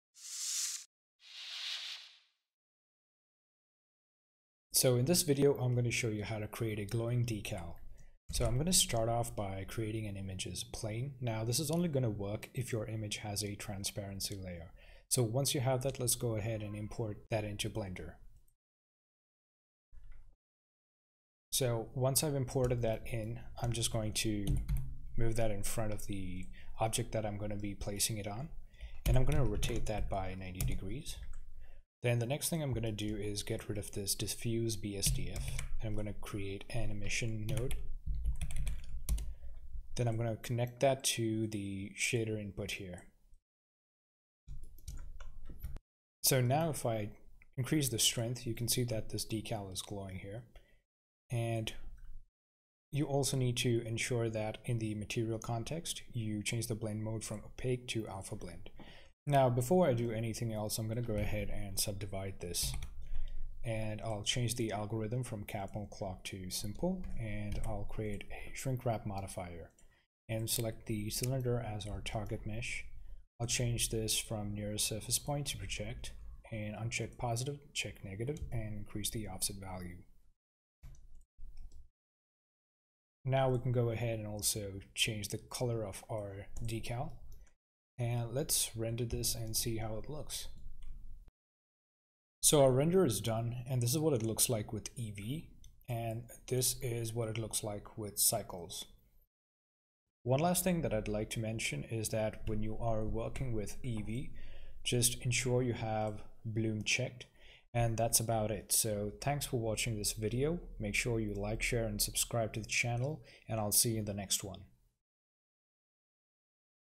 How to Create Glowing Decals in Blender 3D (Under 2 minutes)
In this video, you'll learn how to create glowing decals in blender3D.